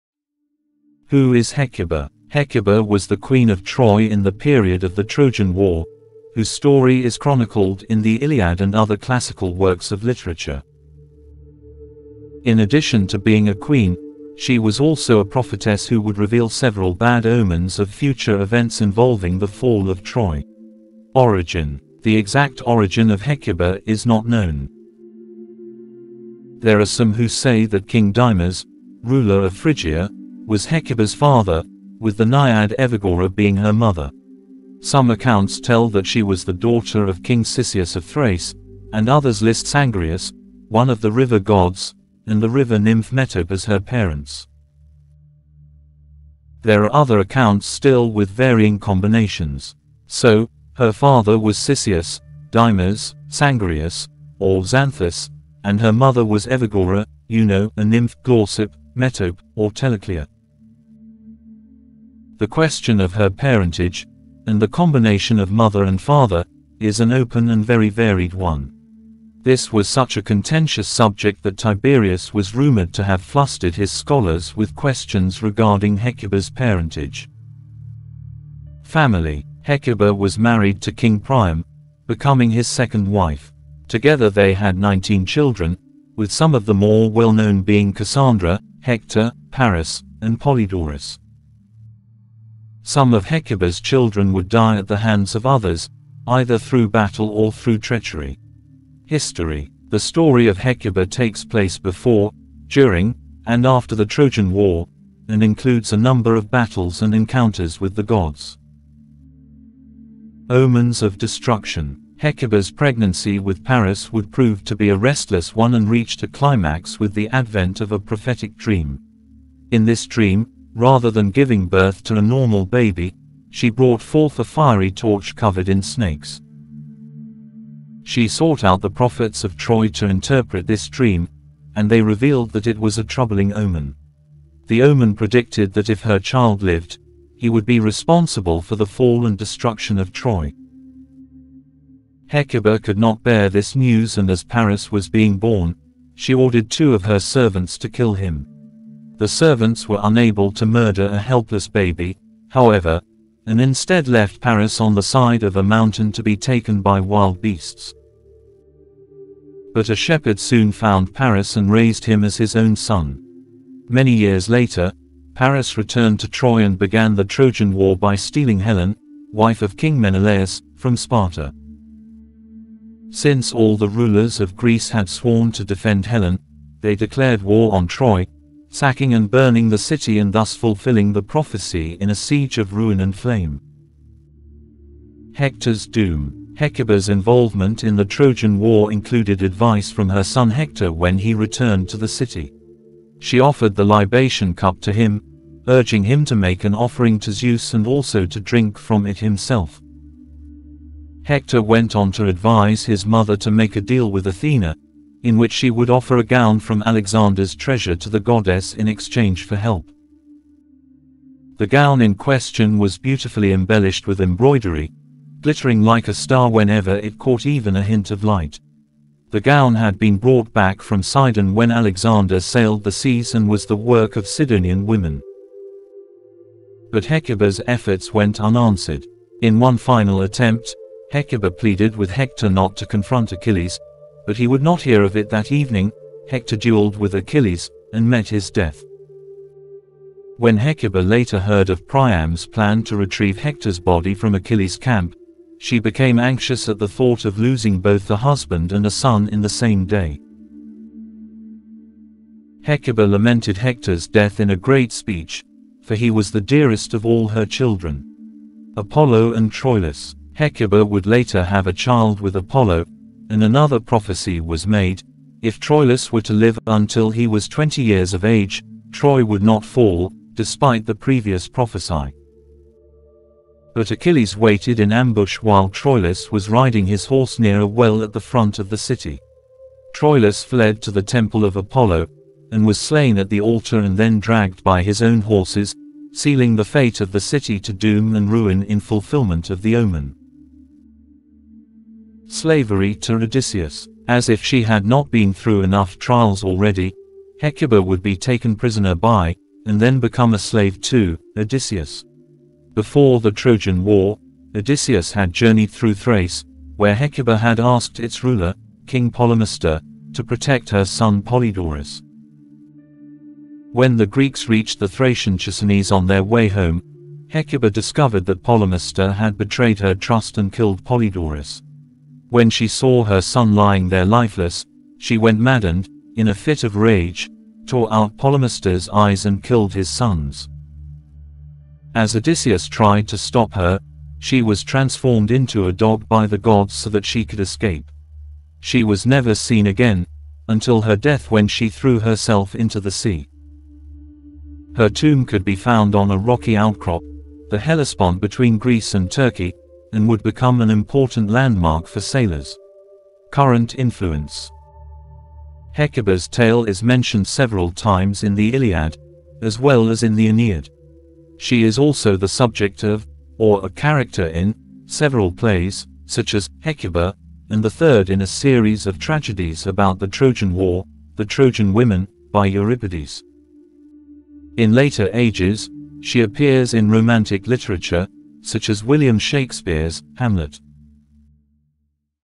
Hecuba: The Queen of Troy in the period of the Trojan War
The exact origin of Hecuba is not known. There are some who say that King Dymas, ruler of Phrygia, was Hecuba’s father, with the Naiad Euagora being her mother. Some accounts tell that she was the daughter of King Cisseus of Thrace, and others list Sangarius, one of the river gods, and the river nymph Metope as her parents. Hecuba was the queen of Troy in the period of the Trojan War, whose story is chronicled in the Iliad and other classical works of literature. In addition to being a queen, she was also a prophetess who would reveal several bad omens of future events involving the fall of Troy.